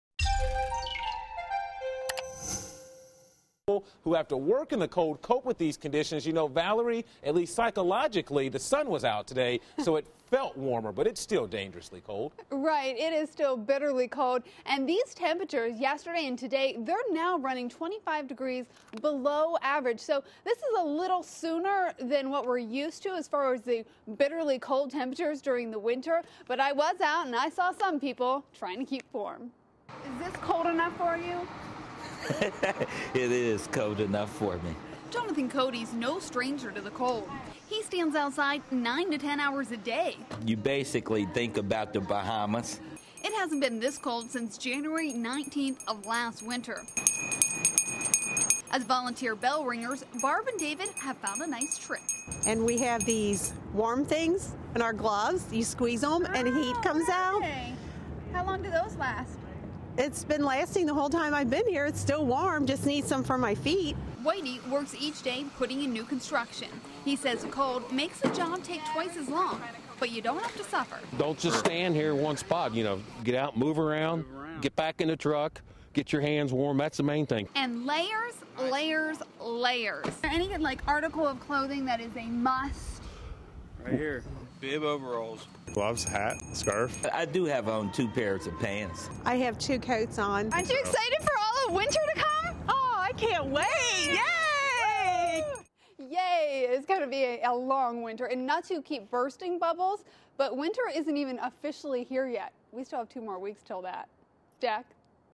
Outdoor Workers Cope With Frigid Cold
If you get chilly just thinking about the walk to your car, imagine if your job kept you outdoors most of the time.